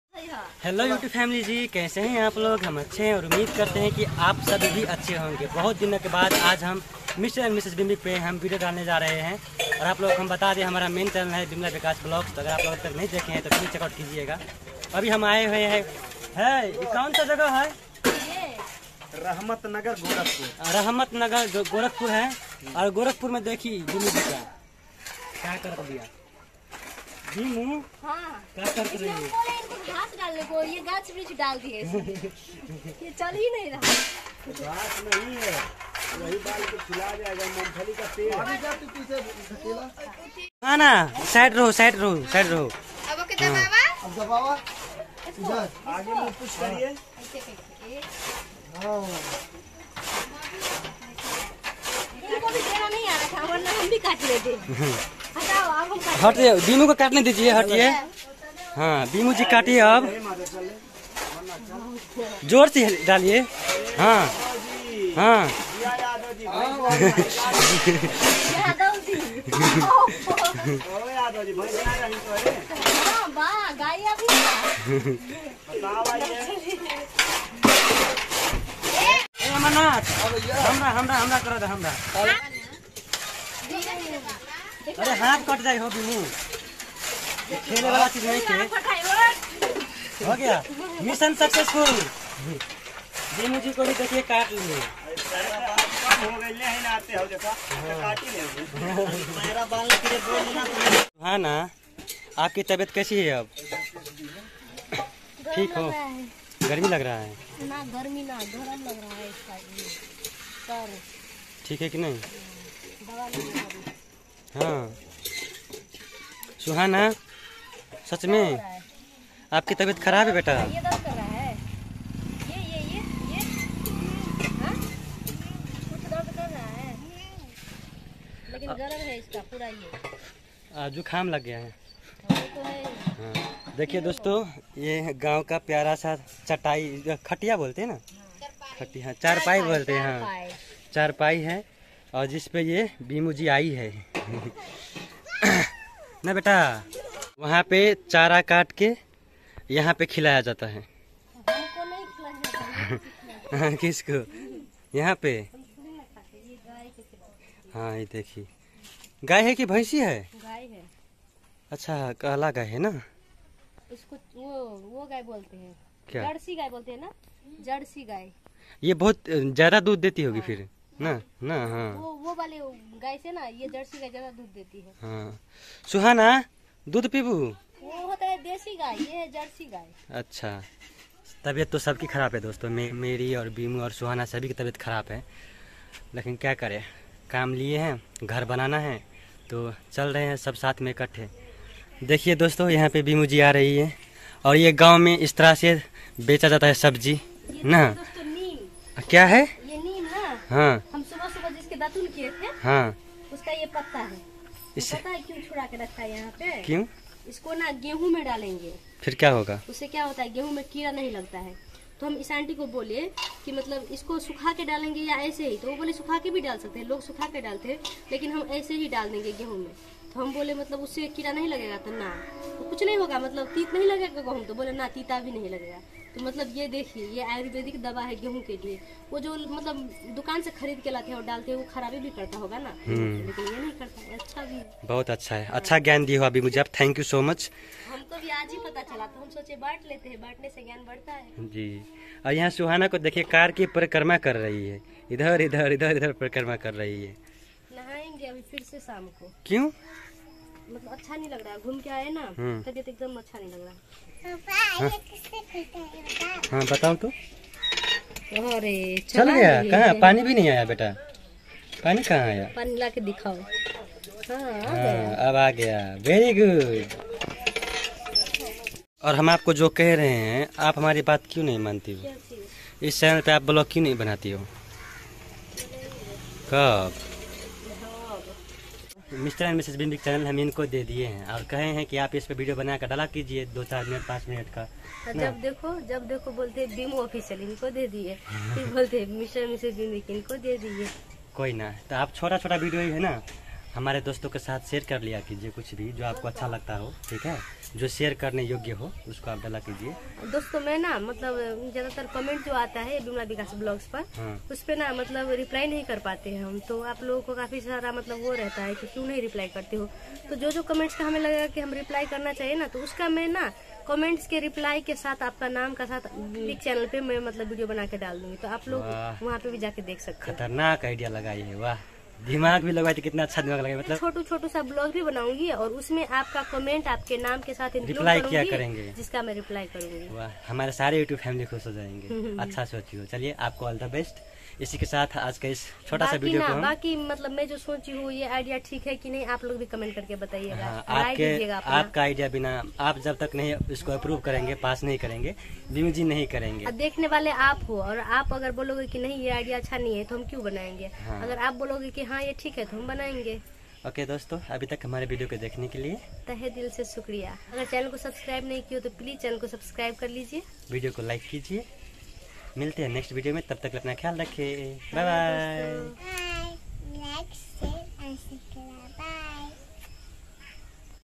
पहली बार बिमु जी अपने हाथ से चारा काट कर गाइ को खिलाइ 🙏
पहली बार बिमु जी अपने हाथ से चारा काट कर गाइ को खिलाइ 🙏 ‎@Mr Vishnuraj Fun